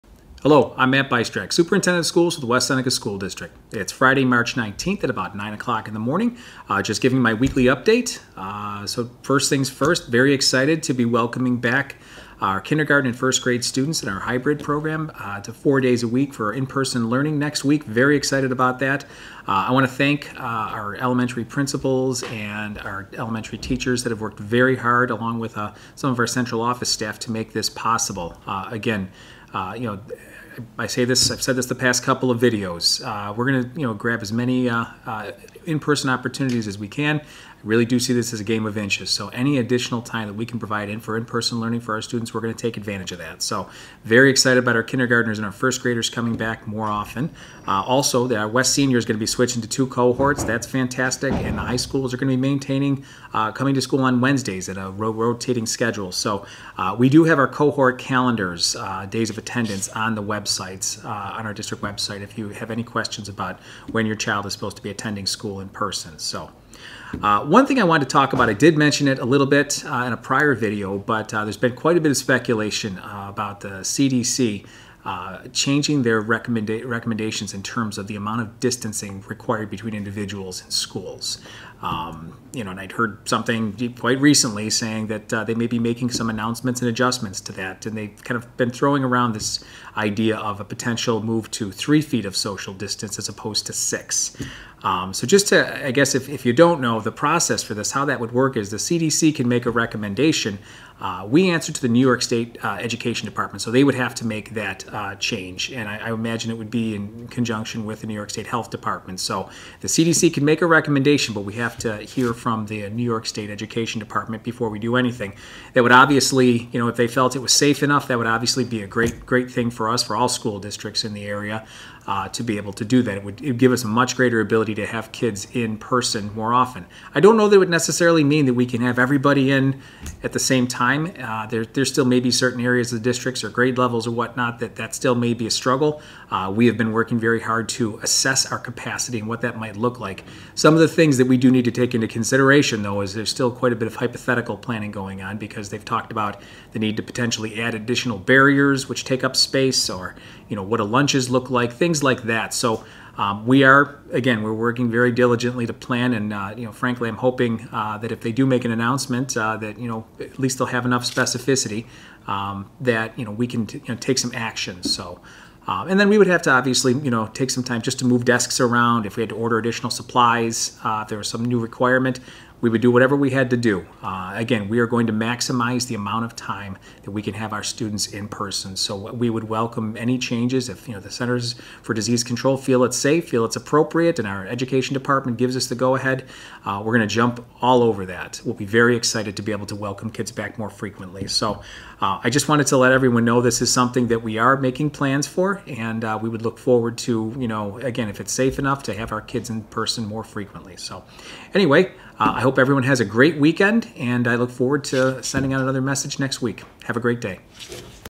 School Community Update, March 19, 2021
Superintendent Bystrak gives his weekly message to the School Community.